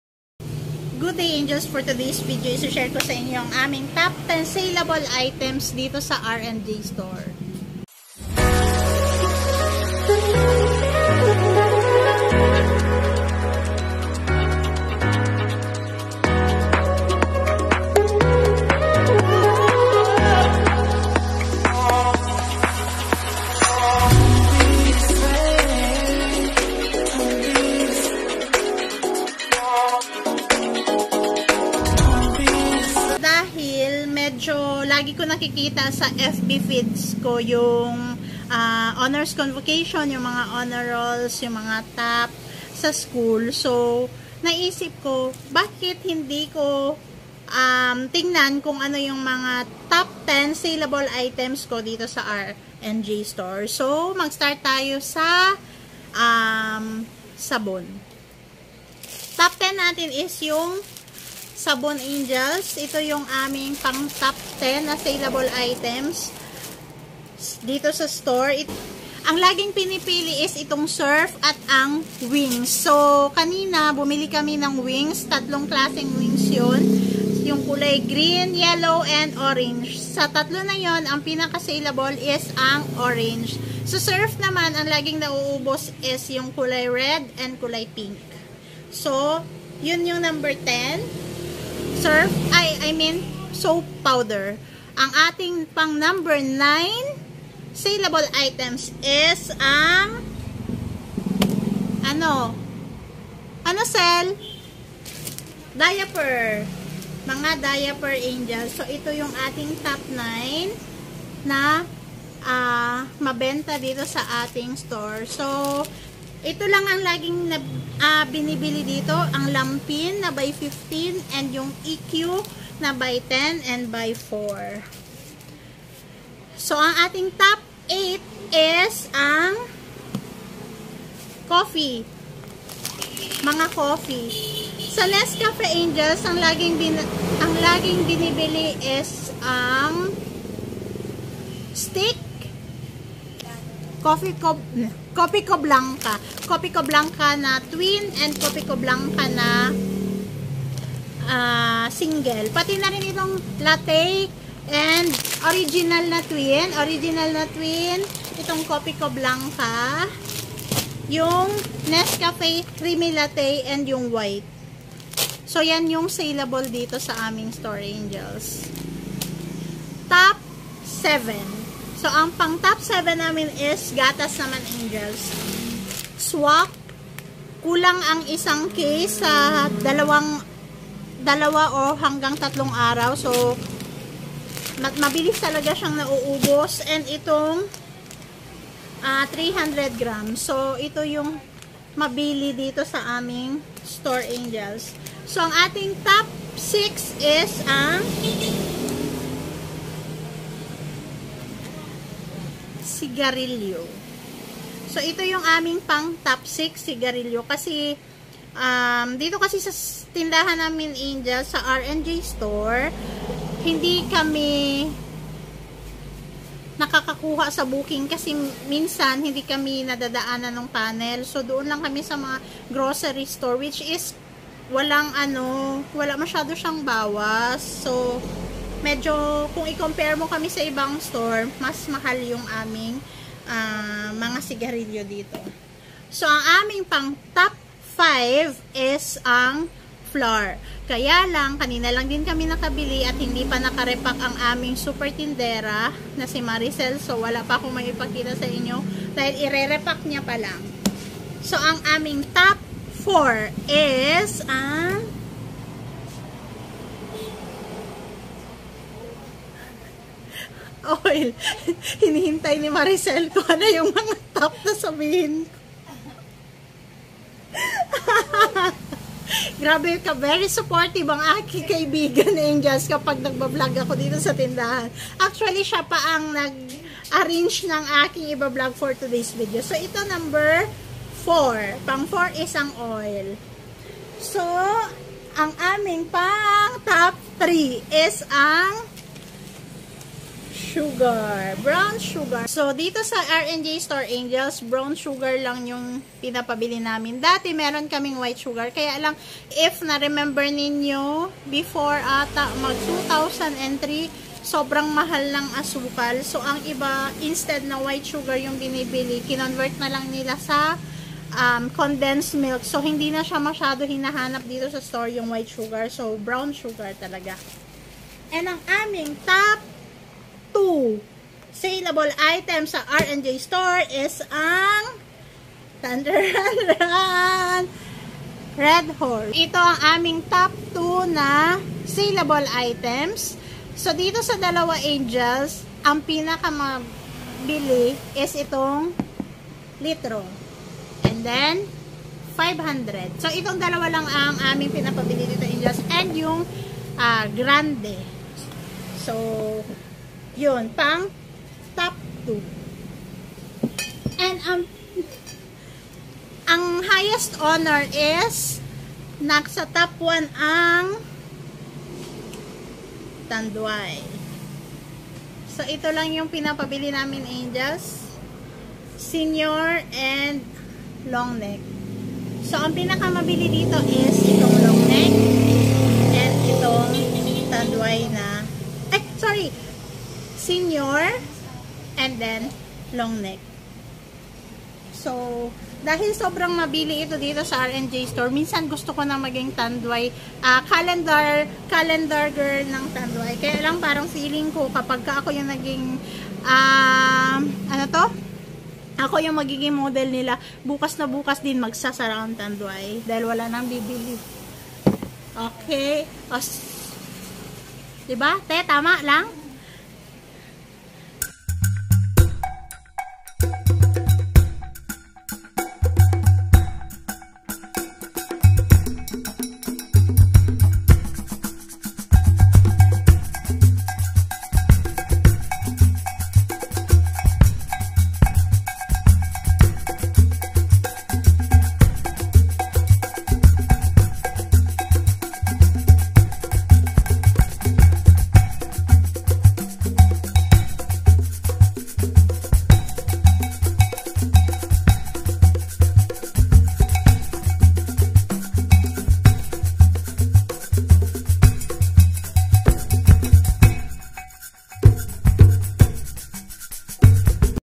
Ano-ano ang madaling maubos sa inyong tindahan? Ito Ang TOP 10 SALEABLE ITEMS sa store ko.
Ano-ano ang mga mabilis maubos na item sa inyong tindahan? Gawan nyo na rin ng top 10 ranking para happy. Thanks sa support angels.😘